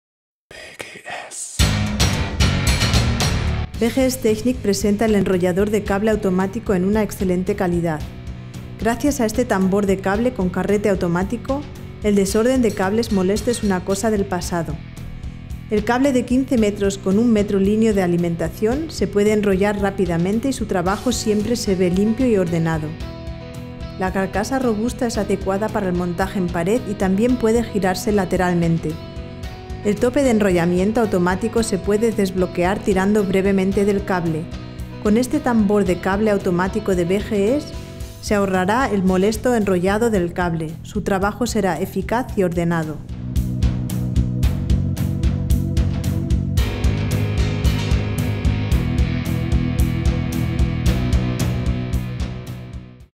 Enrollador de cable automático | 15 m (BGS 3321)
con tambor enrollador de parada automática, se desbloquea tirando suavemente del cable
    con soporte giratorio
    para montaje en pared, incluye soporte de montaje
    sección del cable 3 x 1,5 mm²
- longitud del cable 15 m + 1 m línea de alimentación
- tensión adecuada para 230 V
- dimensiones 295 x 165 x 330 mm
- longitud: 295 mm
- anchura: 165 mm
- altura: 330 mm
- peso: 4,5 kg